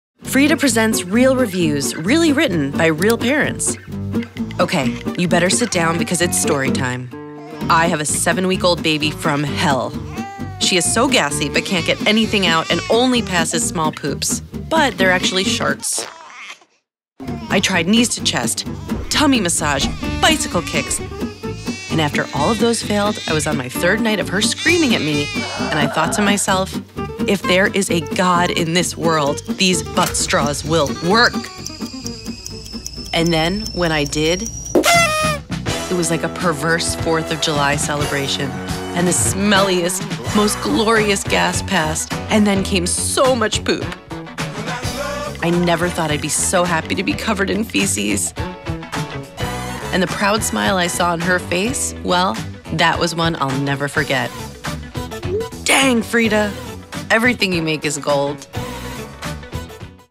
Frida Baby Presents Real Reviews: Windi Edition
Real Frida reviews, reenacted by real parents. In this episode, we learn that a well-executed shart is a thrill known only to parents who use Windi the Gaspasser. A gassy, constipated baby, plus the Windi = glorious relief, sometimes with a side of s%*t.